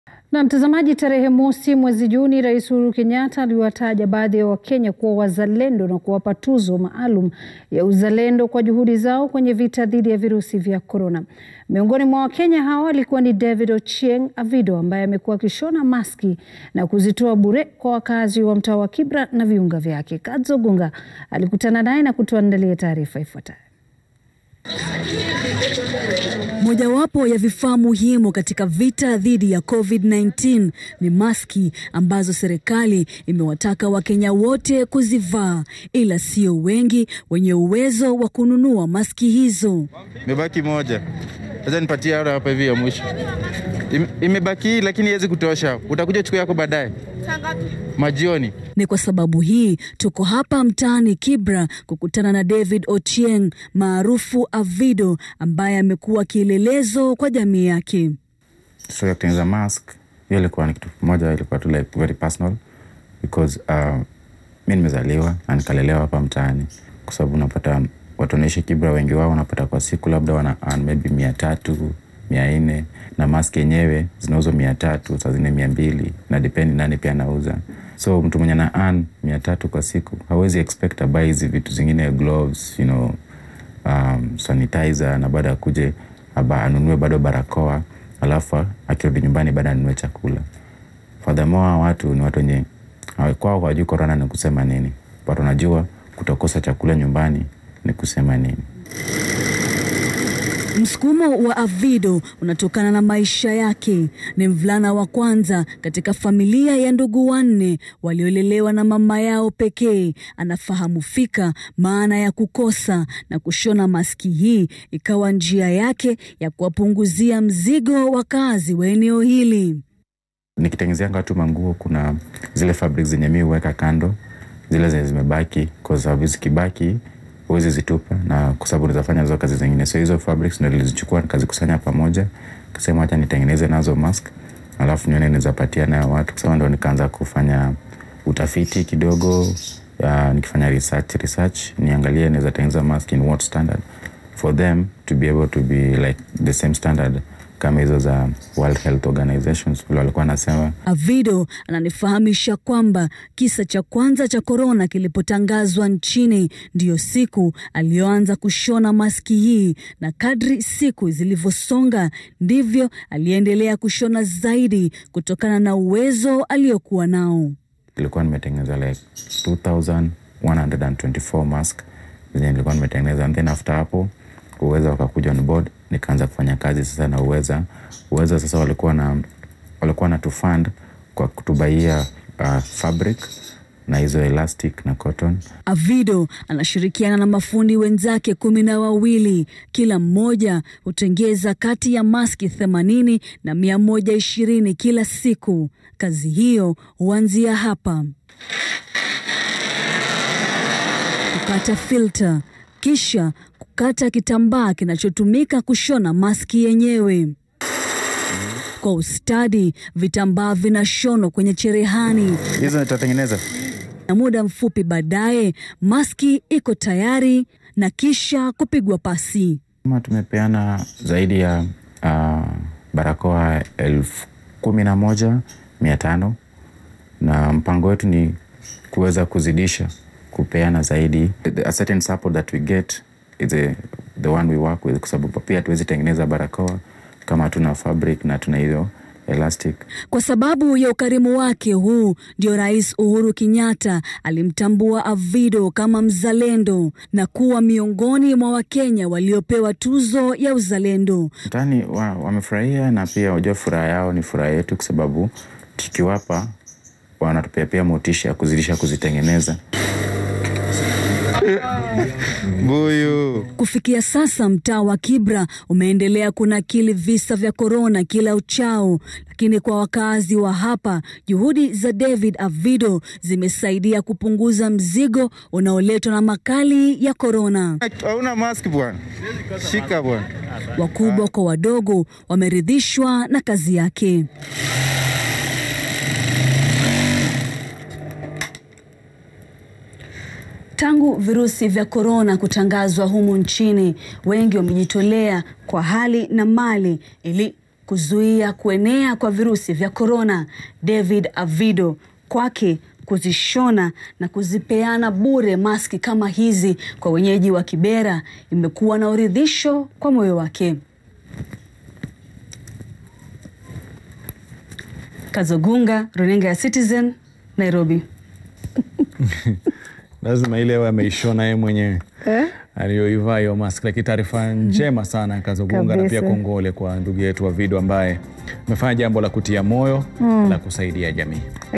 David Ochieng maarufu Avido anatengeza maski na kuwapa wakaazi Kibra
Tarehe mosi mwezi Juni, Rais Uhuru Kenyatta aliwataja wakenya kadhaa kuwa wazalendo na kuwapa tuzo maalum ya uzalendo kwa juhudi zao kwenye vita dhidi ya virusi vya corona. Miongoni mwa wakenya hawa alikuwa ni david ochieng avido ambaye amekuwa akishona maski na kuzitoa bure kwa wakaazi wa mtaa wa kibra na viunga vyake.